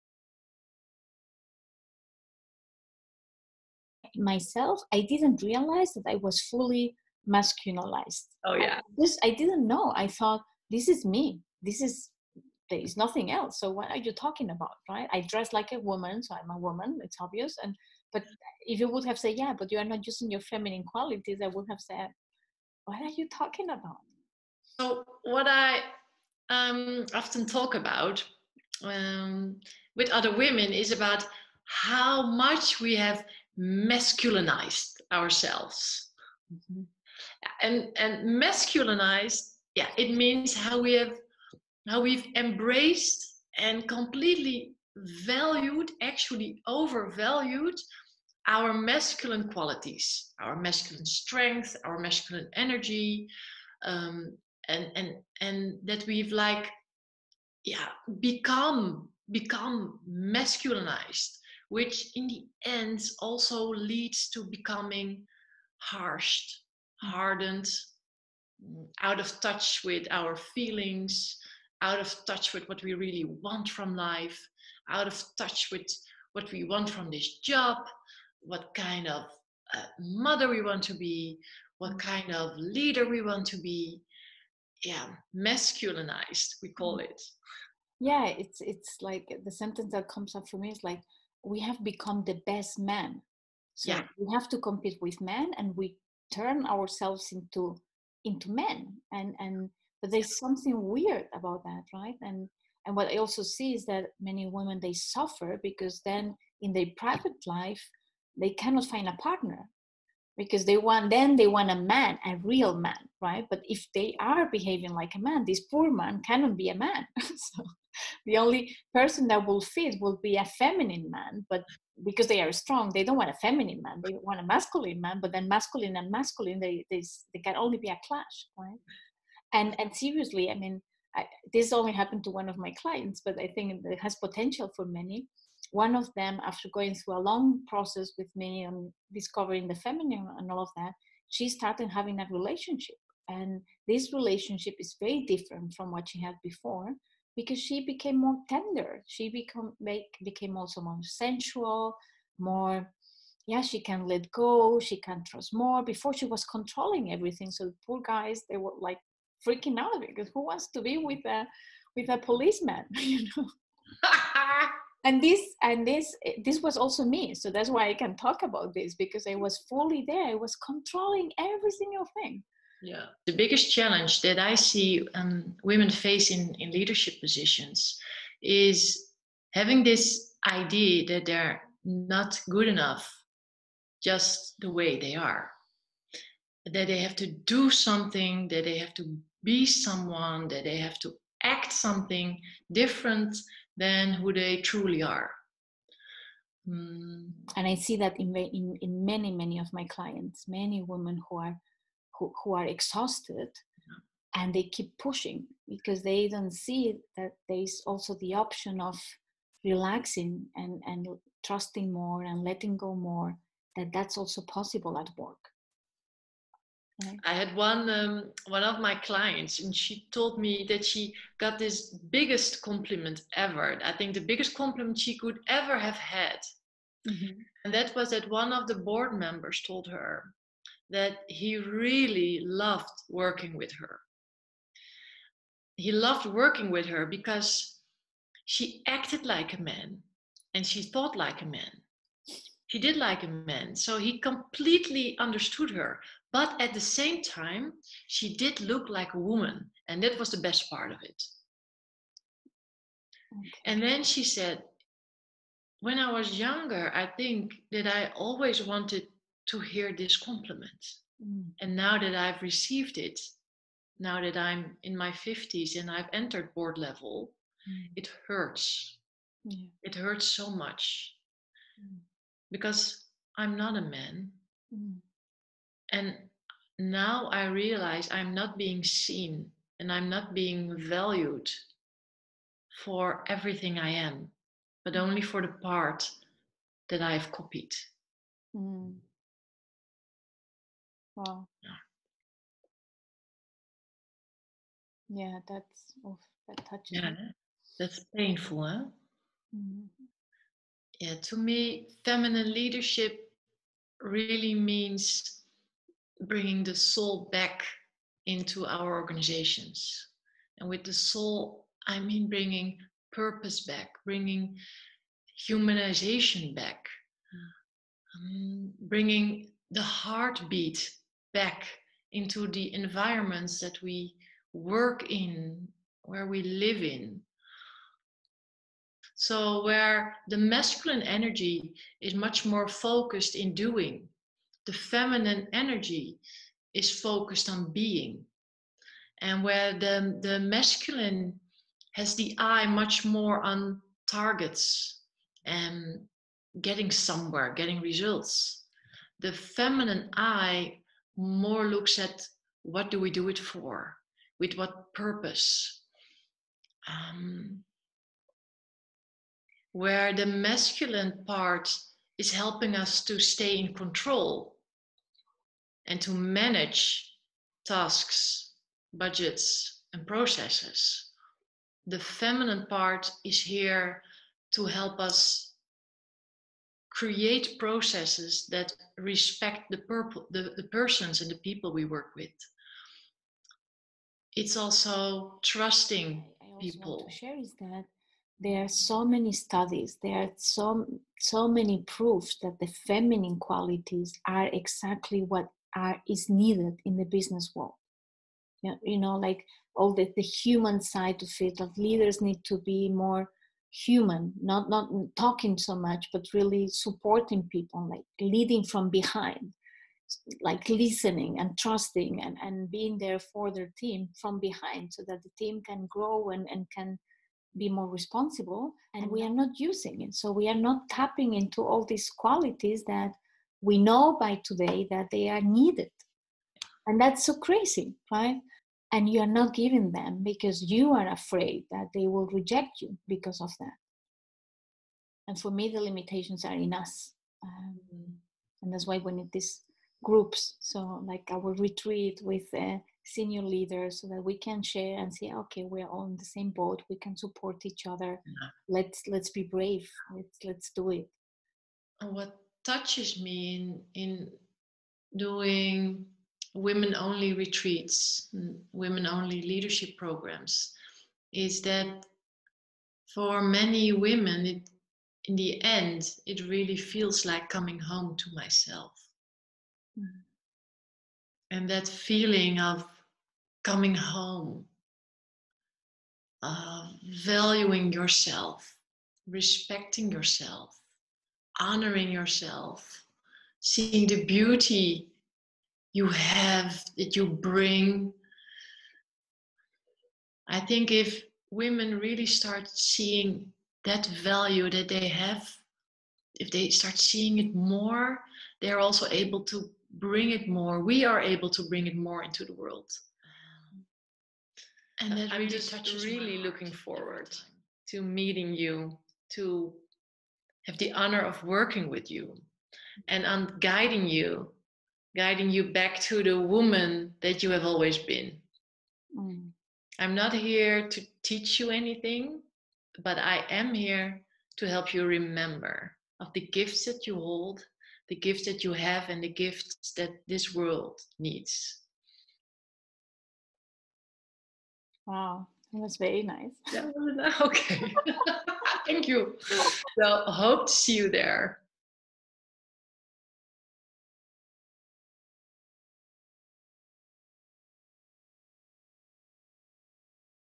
Feminine Leadership by Sonia Herrero & Carla Clarissa
Learn about what it means to tap into your feminine qualities as a women leader. Find more information about the Feminine Leadership retreat: